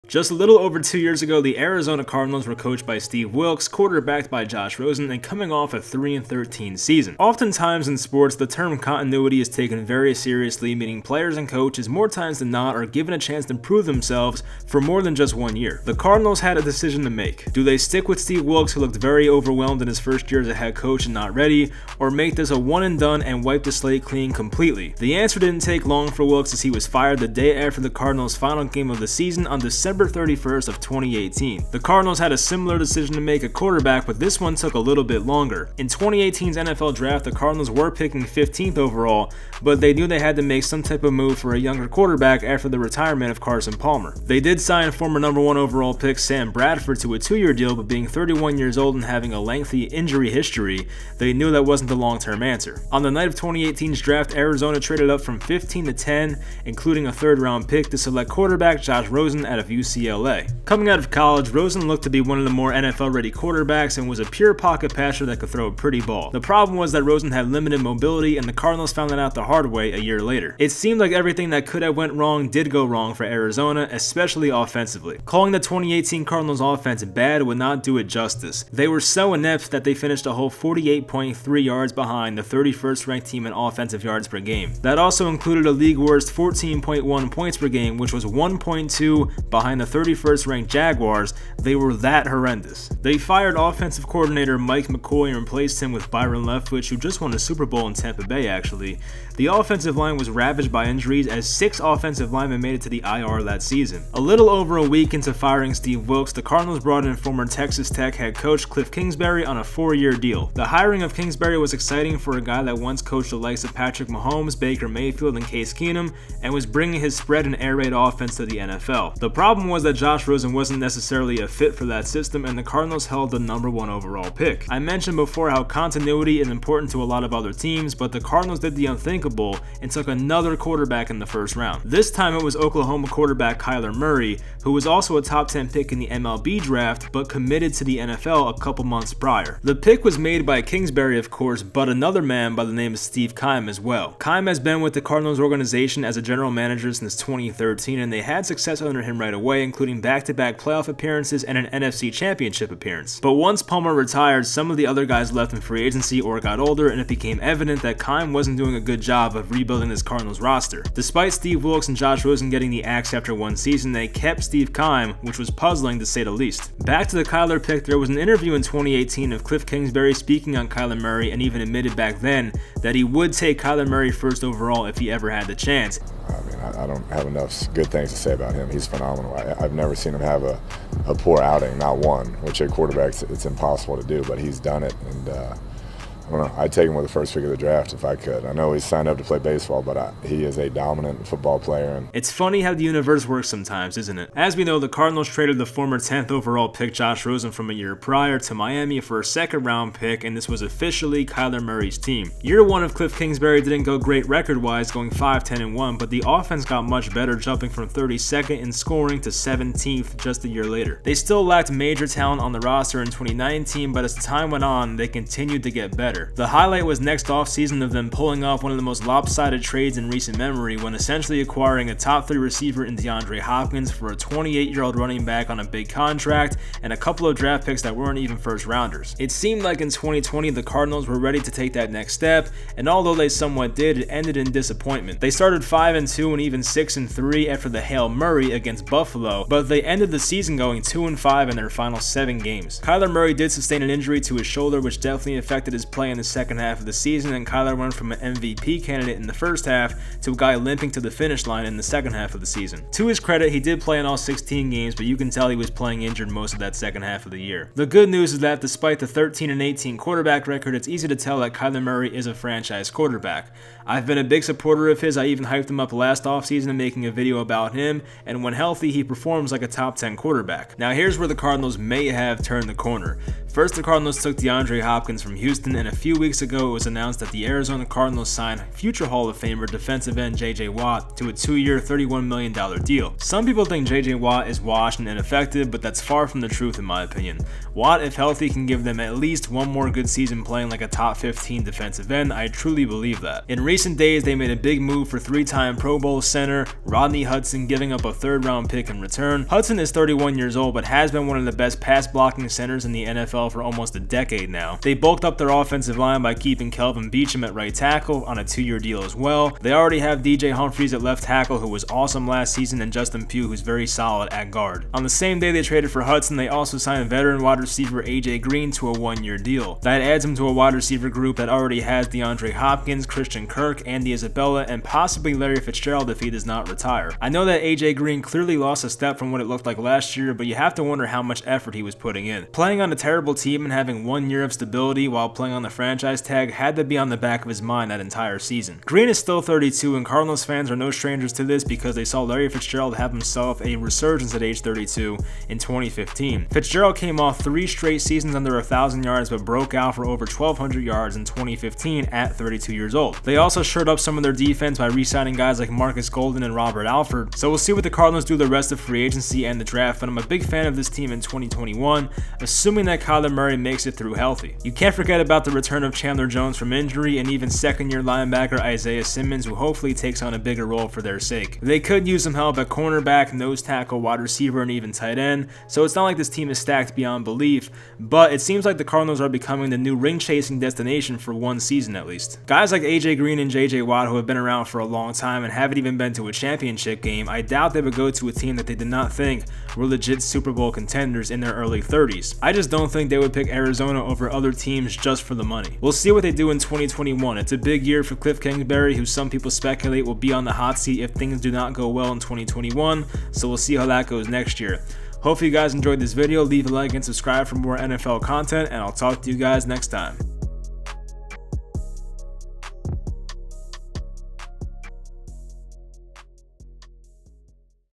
How The Arizona Cardinals Went From 3-13, To Super Bowl Contenders in 3 Years
Going over the Arizona Cardinals from 2018-present day. From making a first round mistake at QB, to selecting a QB the next season and building a very good roster in 2021